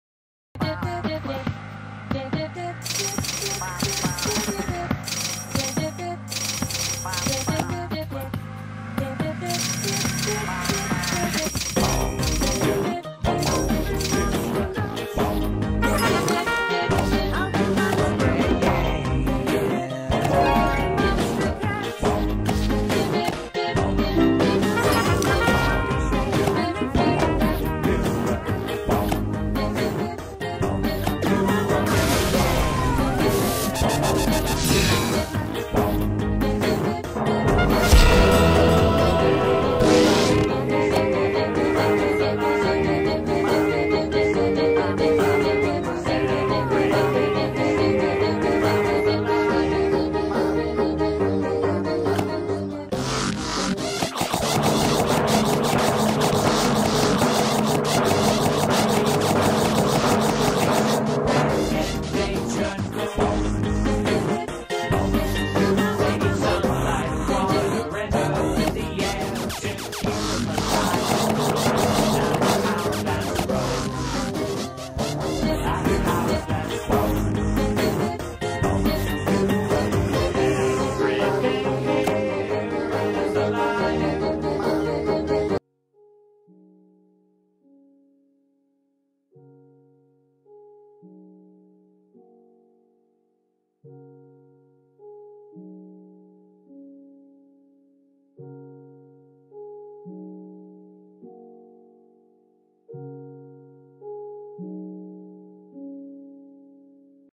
getting my first wubbox and im back
got first wubbox and came back in the same vid!!! 😤😤🤯🤯